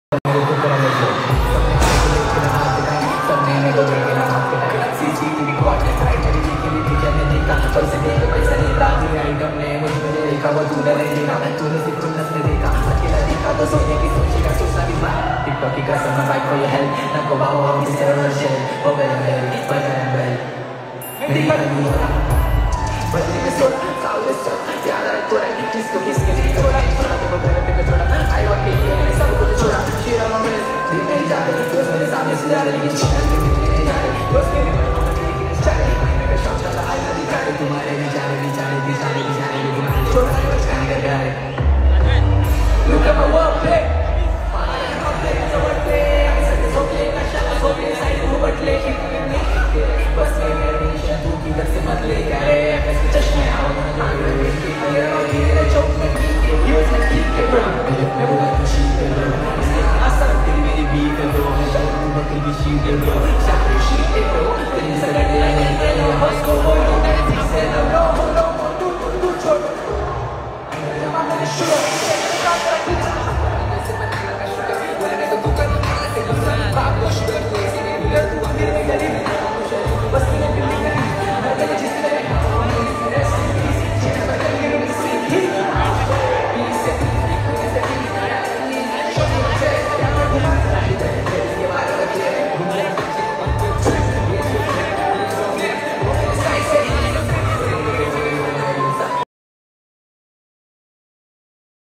MC STAN Mumbai Live Show 2023
bilkul raw live recording hai maam.
big up to @haqsestan6969